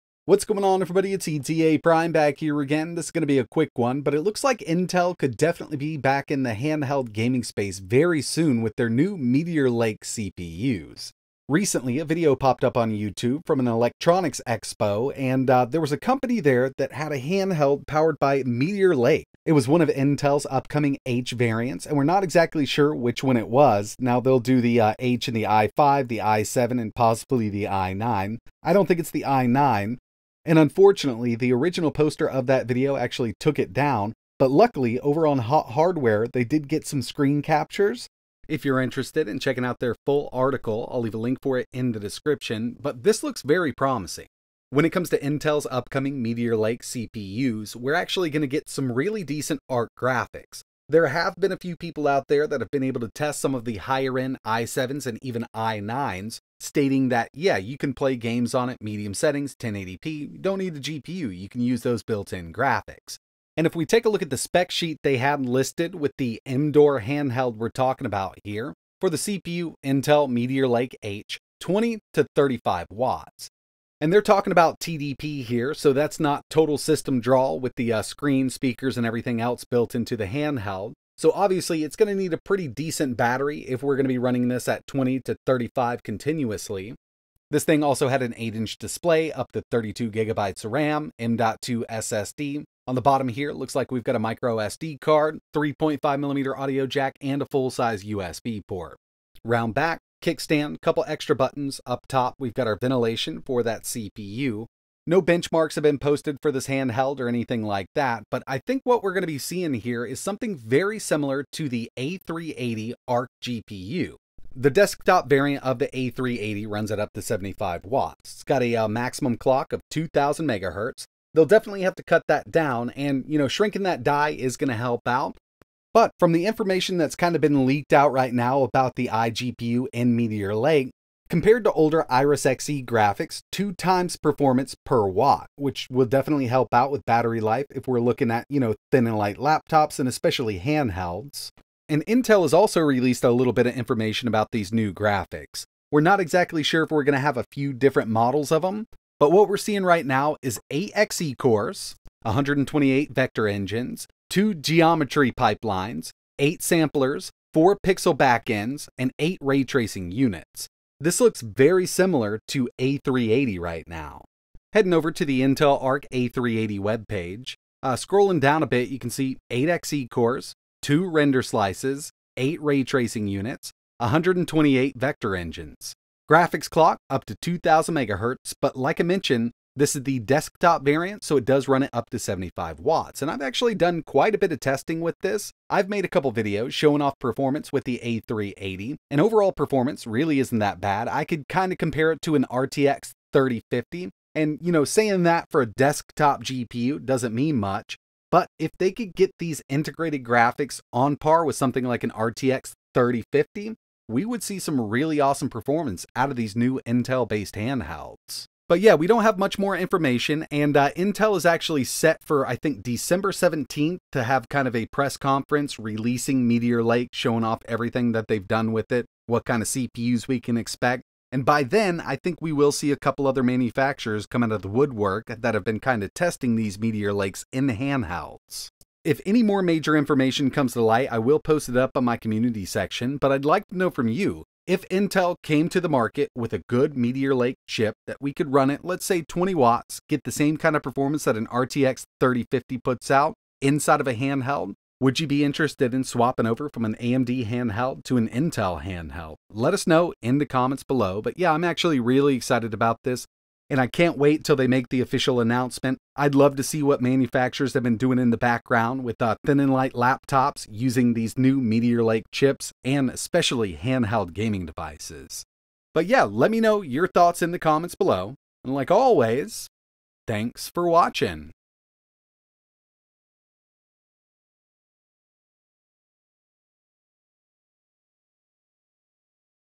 Intel Is Coming Back To The Handheld Game With Big iGPU Power!
Looks Like Intel will be getting back in to the handheld market with thir upcoming Meteor Lake CPUs! Recently Emdoor showed off the First Handheld Gaming PC With An Intel Meteor Lake CPU and from the news so far it might give AMD a Run for its money with an all new ARC iGPU!

ETAPRIME
12400 Wake Union Church Rd PMB 239